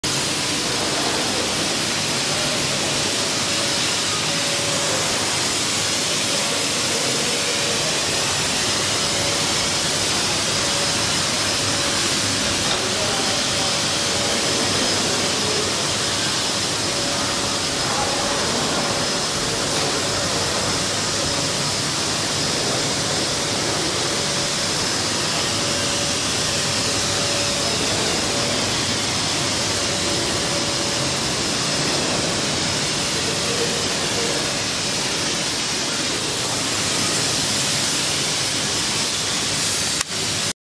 Inflatable medical emergency tent for novel coronavirus
The inflatable medical tent is 7 meters by 4.5 meters by 2.5 meters. The inflatable emergency tent is made of 0.9mm PVC tarpaulin. The hospital tent is waterproof and flame-retardant. The ermegency camping tent is inflated by eletric pump. The rescue tent is portable.